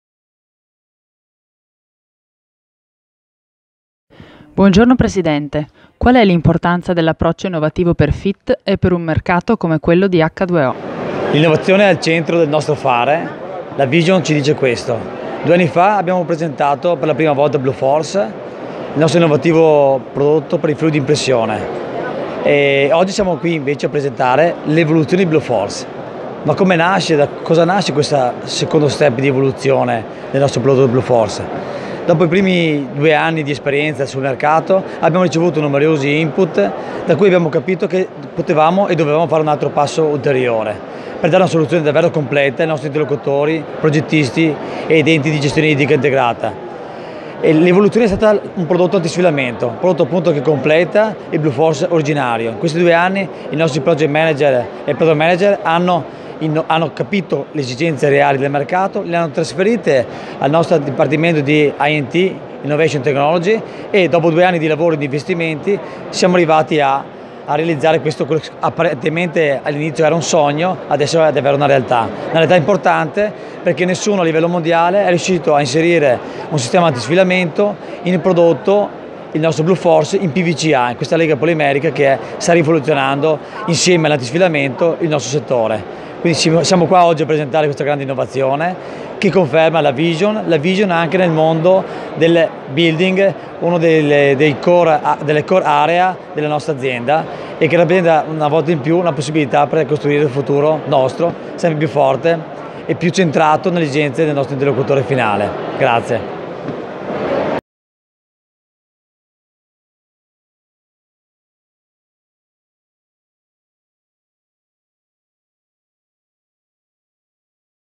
Alessandro Mezzalira (CEO FITT) - ACCADUEO 2018
Innovazione al centro. Alessandro Mezzalira (CEO FITT) racconta il percorso che ha portato alla nascita del rivoluzionario sistema antisfilamento FITT Bluforce RJ.